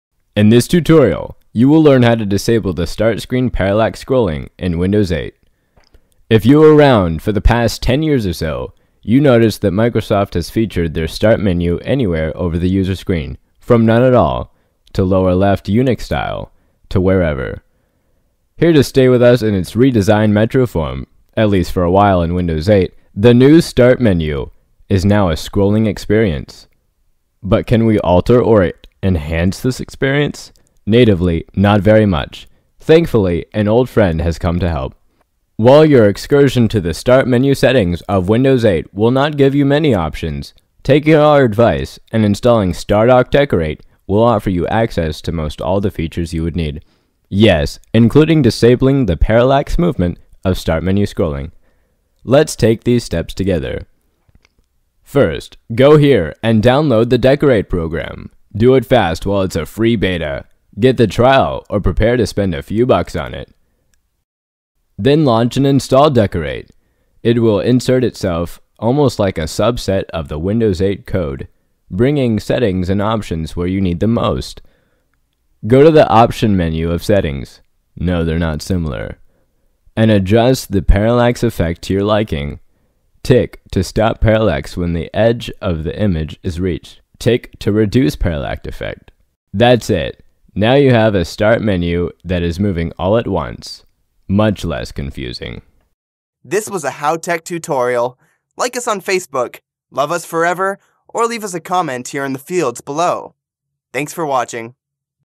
How to Disable Start Screen Parallax Scrolling in Windows 8
In this tutorial, you will learn how to disable the start screen parallax scrolling in Windows 8.

If you were around for the past ten years or so, you've noticed that Microsoft has featured their start menu anywhere over the user screen, from none at all, to lower left, to "Unix-style", to wherever. Here to stay with us in its re-designed Metro form, at least for a while in Windows 8, the new start menu is now a scrolling experience.

But can we alter or enhance this experience? Natively, not very much. Thankfully, an old friend comes to help. While your excursion to the start menu settings of Windows 8 will not give you many options, taking our advice and installing Stardock's Decor8 will offer you access to almost all the features you would need -- yes, including disabling the parallax movement of start menu scrolling. Let's take these steps together.

Step 1.  and download the Decor8 program. Do it fast while it's a free beta, get the trial or prepare to spend a few bucks on it.

Step 2. Then launch and install Decor8. It will insert itself almost like a subset  of Windows 8 code, bringing settings and options where you need them most.

Step 3. Go to the "Options" menu of "Settings" (no they're not similar) and adjust the parallax effect to your liking. Tick to stop parallax when the edge of the image is reached. Tick to reduce parallax effect.

Result: That's it, now you can have a start menu that is moving all at once -- much less confusing.